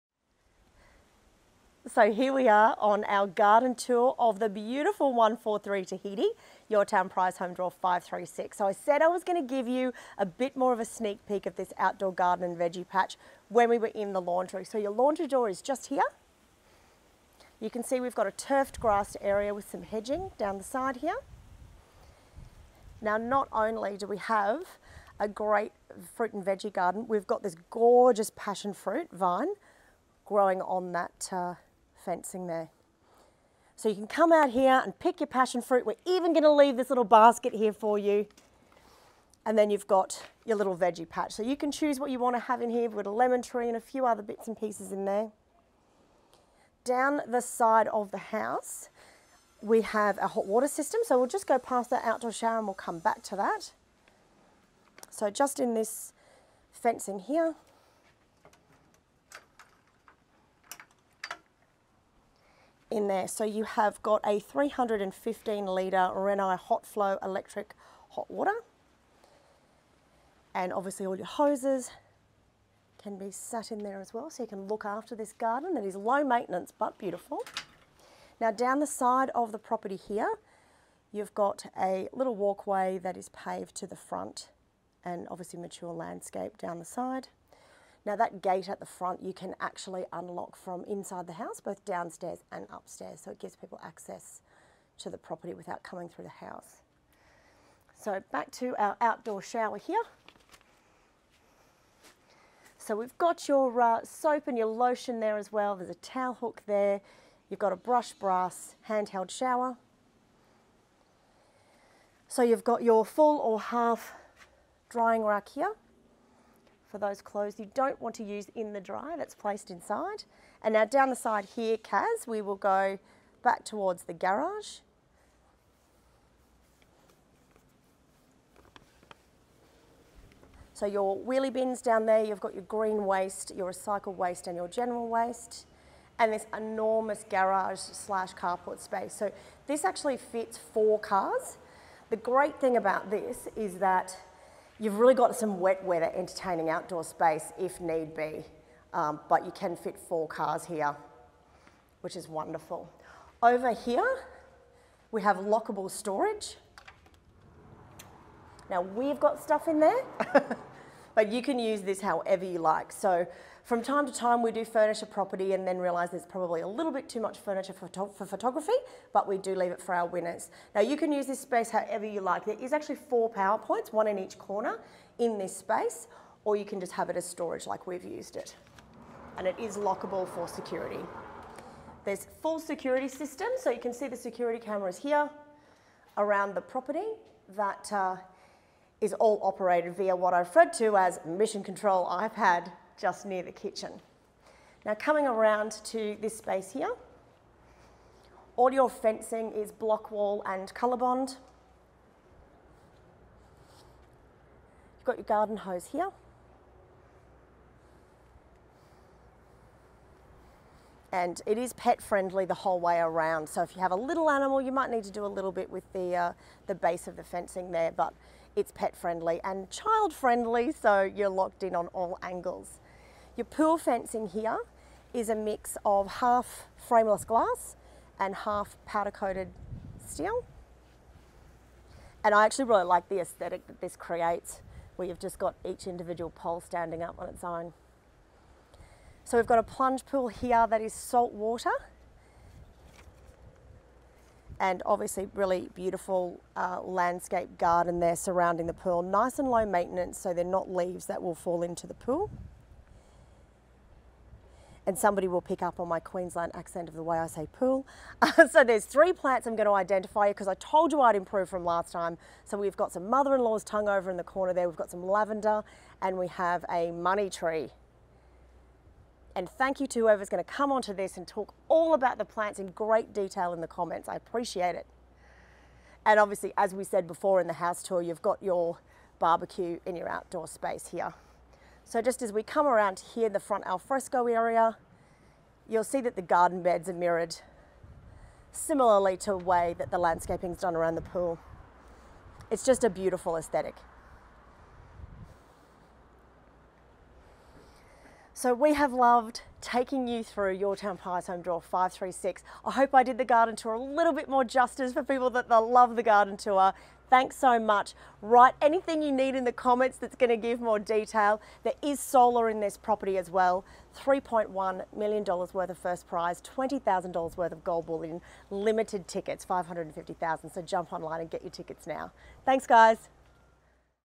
GARDEN TOUR | Palm Beach Prize Home | Draw 536
Join Tracey as she takes you through the gardens at our Palm Beach Prize Home 🥕🏡 You could WIN this gorgeous home with a $15 ticket